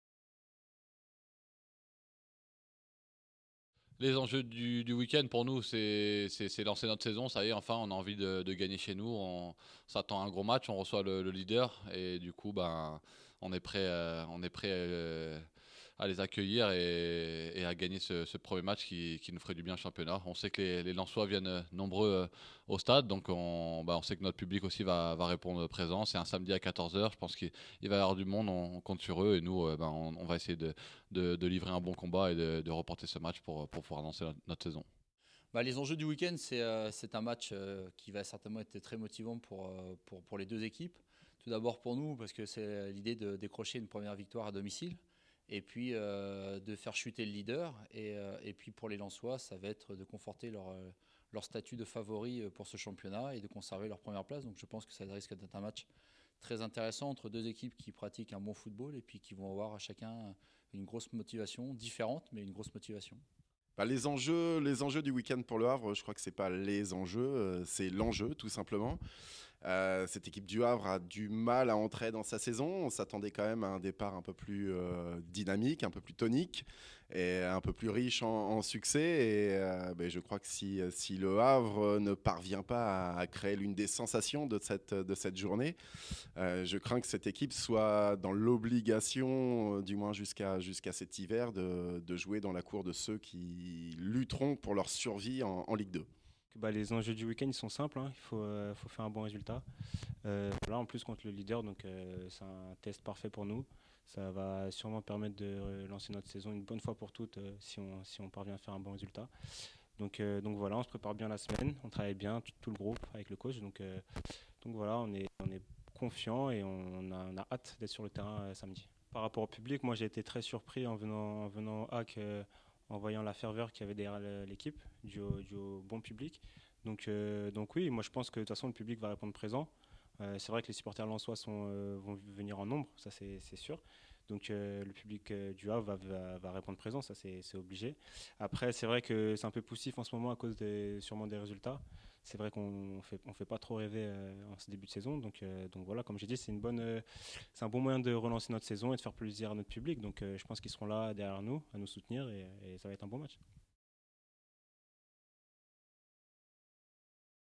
HAC - Lens: les enjeux du week-end...
Ce samedi, les Ciel & Marine accueillent Lens au Stade Océane. Nous avons demandé à Yohann Rivière, Tristant Dingomé, François Seguin (entraîneur des gardiens) et Benoit Dockèle (journaliste à la Presse Havrais) quels étaient les enjeux du week-end pour le HAC.